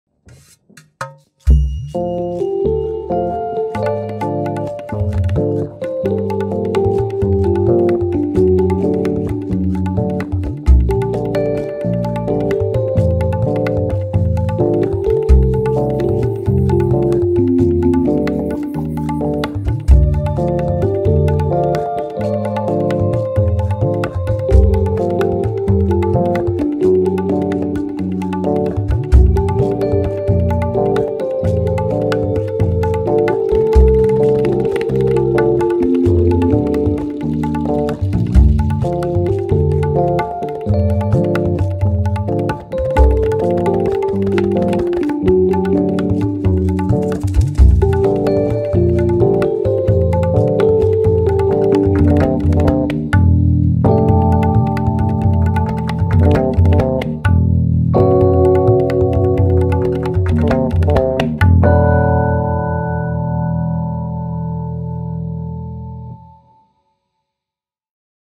Ronan Skillen and Shane Cooper - Quarantine Collabs #02 (Music Edition)
I've been working on some music collaborations remotely while in lockdown in South Africa. This one is with multi-instrumentalist Ronan Skillen on percussion